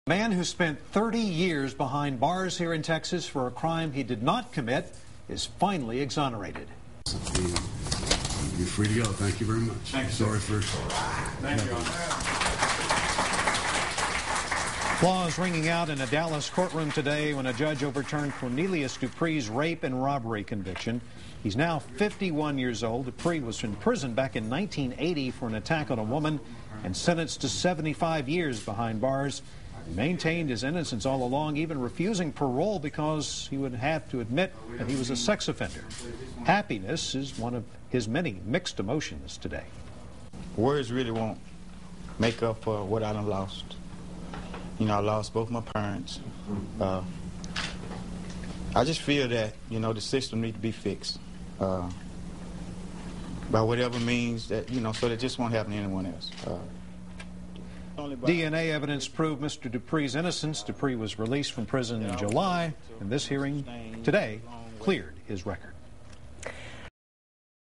Man released after wrongly imprisoned 30 years
DNA proved the innocence of Cornelius Dupree, Jr., in a rape and robbery conviction, and he was released from prison Tuesday after being locked up for 30 years.